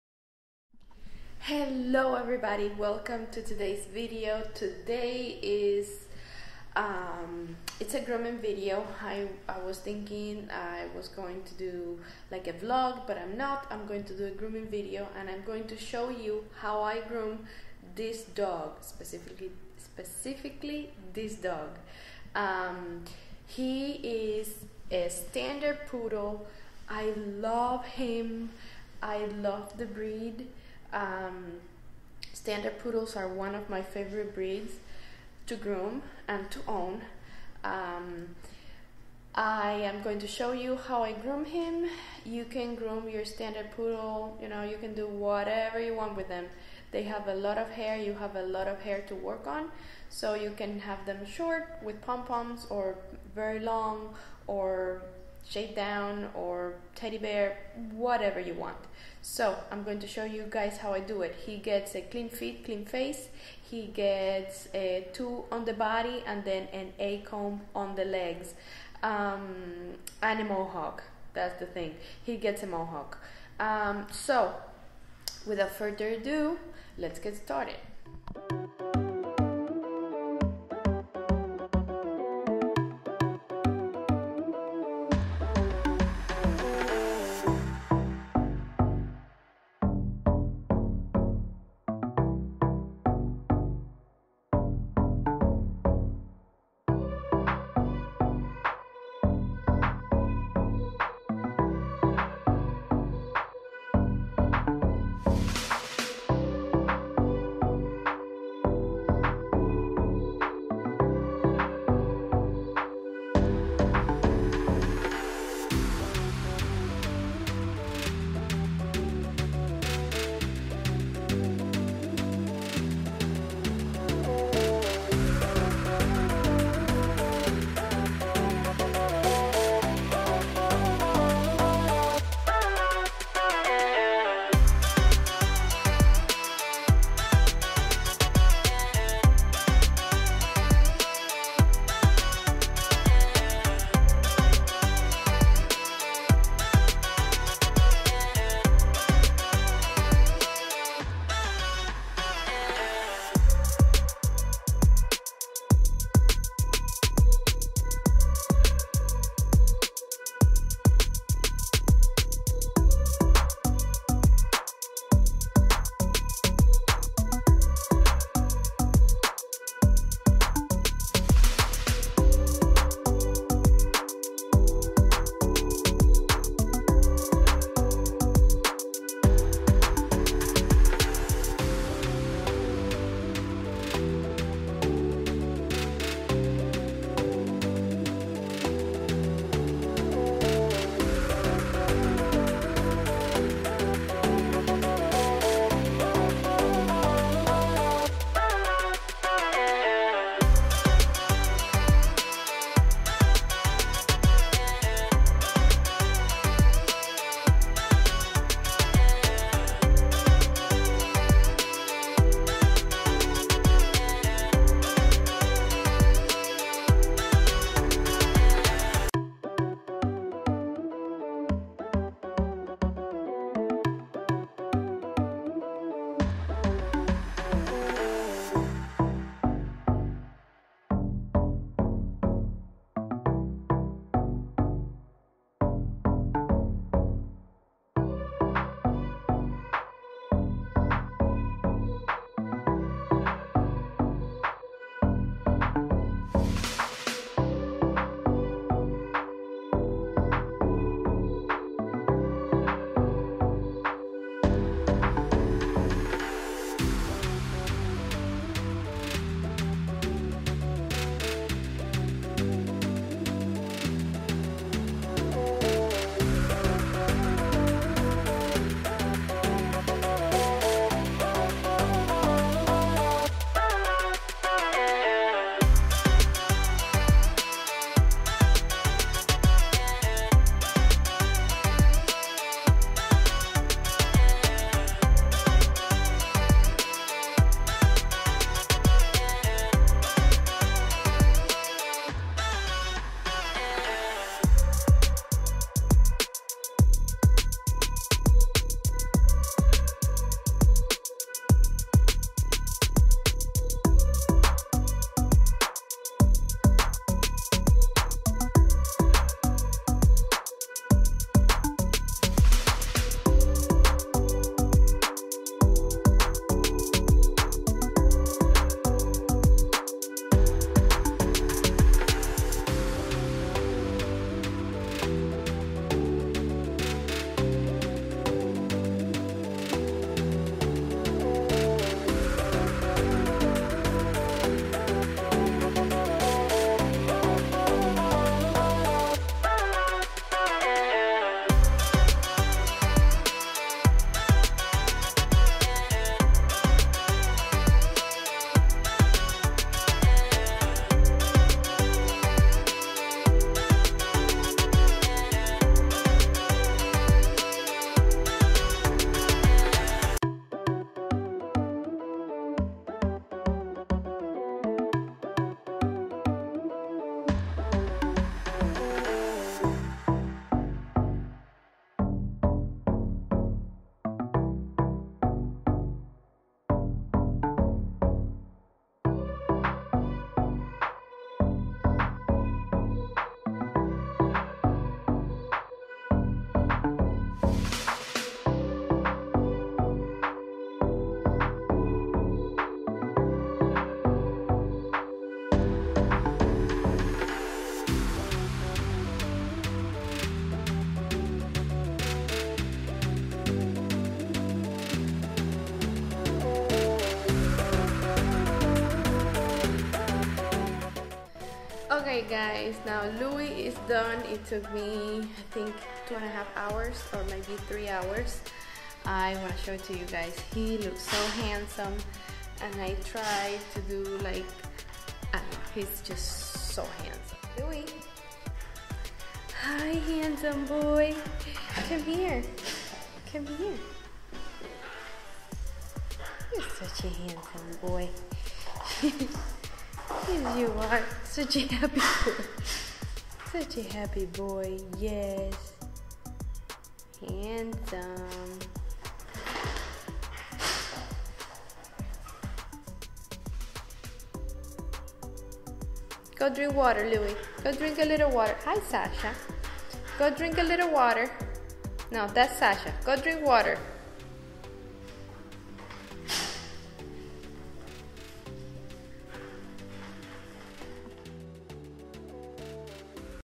Standard Poodle pet trim alternative/ How to groom my standard poodle/ Beautiful Spoo summer clip
In this video, I will show you a beautiful haircut for your Standard Poodle. This is an easy way to groom your standard poodle. You can show this video to your groomer or give them the instructions.
If you want a regular topknot and not a Mohawk, that can be done easily.
I used a 1comb on the body, A comb on the legs, clean face and feet. Then I scissor all over and left a topknot and the crest lightly blended (that’s what makes it a Mohawk)
I forgot to mention that I also did shave his ears with a 10 on the outside and 40 the inside.
This looks like a modified German clip.